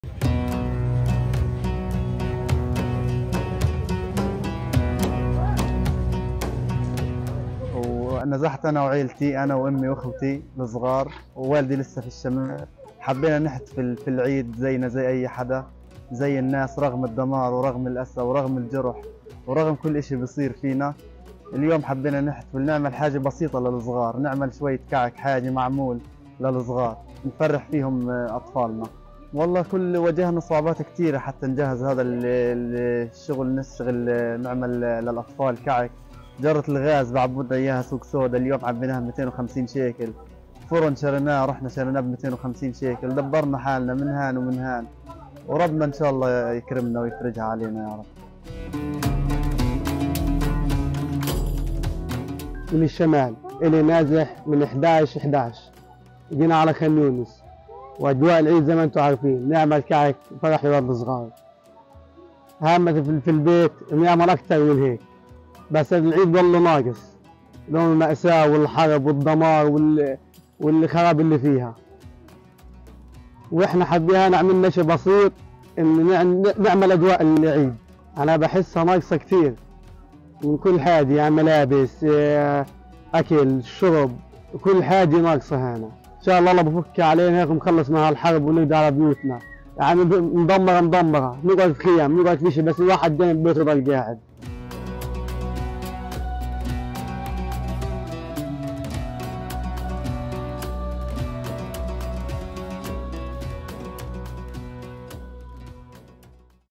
مبادرة لعمل الكعك والمعمول في قطاع غزة رغم الحرب | بي بي سي نيوز عربي
أحمد وإبراهيم نازحان من شمال غزة إلي خانيونس، قررا أن يقوما بعمل الكعك والمعمول ليدخلوا السرور علي أسرتيهما وعلي الأطفال في المنطقة.

وليد حسن تحدث إليهما وسألهما عن الصعوبات التي واجهاها في إعداد كعك العيد